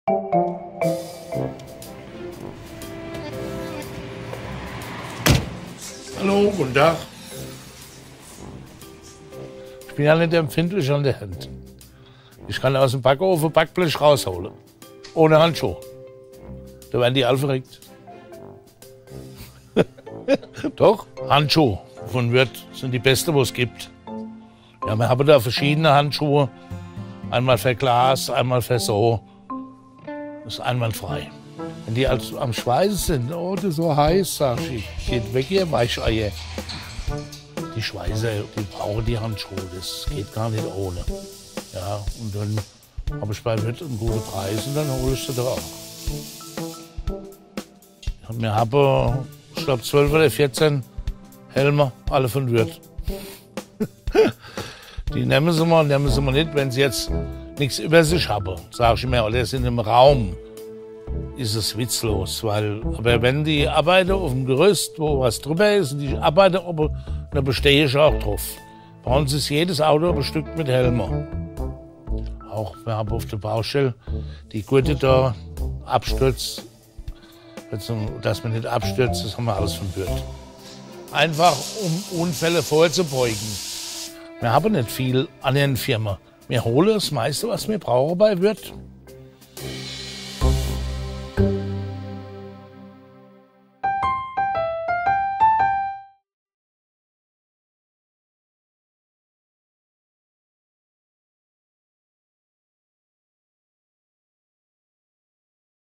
Würth – Das Original. Made in Germany: Heinz und der Arbeitsschutz
Heinz Metzger, der erste Handwerker unserer neuen Serie „Das Original – made in Germany“, fasst ein Backblech aus dem Ofen ohne Handschuhe an. Warum er trotzdem auf PSA von Würth schwört und dem professionellen Handwerk wärmstens Handschuhe, Helme und Gurte von Würth als Arbeitsschutz empfiehlt, erzählt er hier. Wie immer heiter und unterhaltsam…